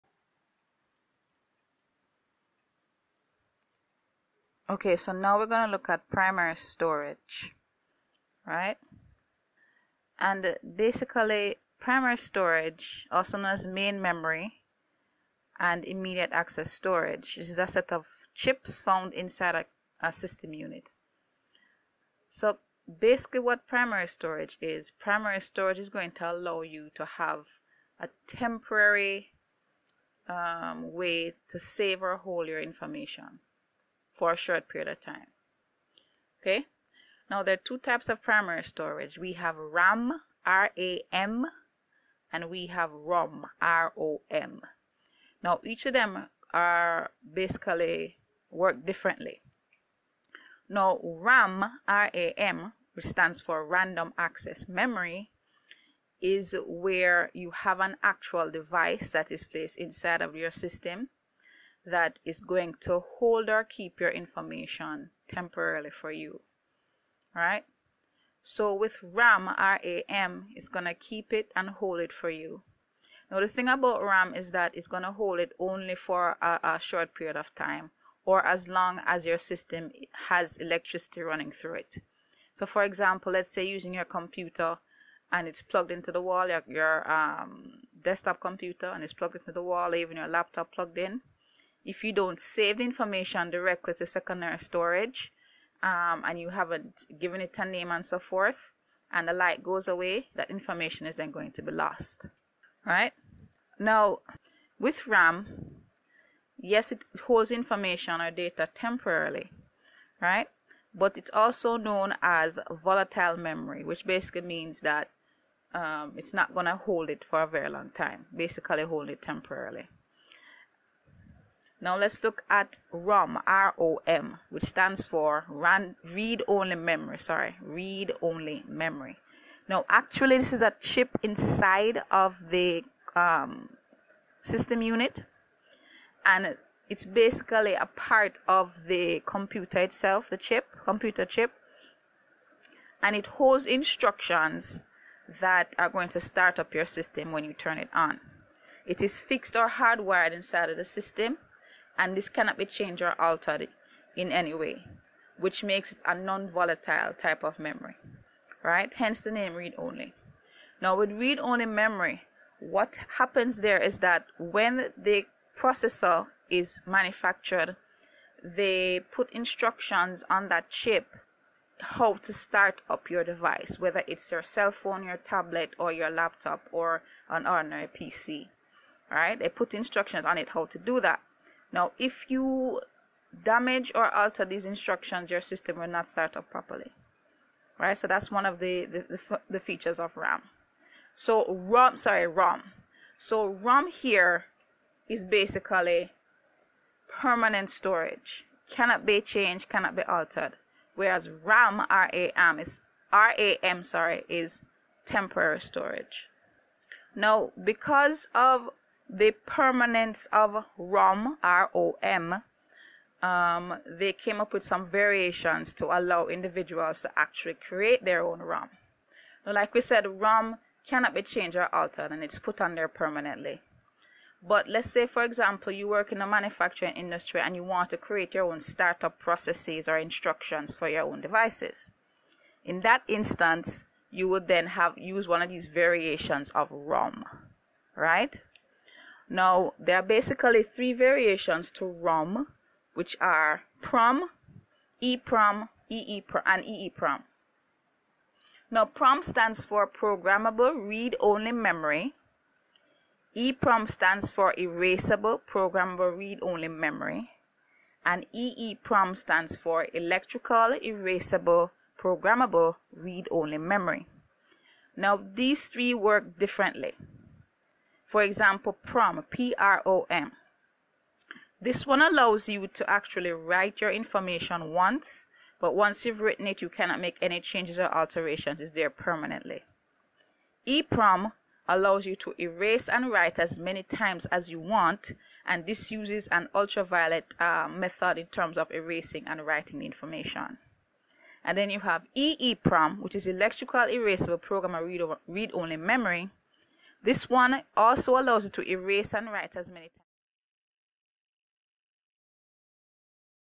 Primary Storage
This video is about Primary Storage, it is the second topic covered in the Information Technology syllabus for CSEC.  This is the first of two videos on this topic. It was created using Powtoon -- Free sign up -- Create animated videos and animated presentations for free.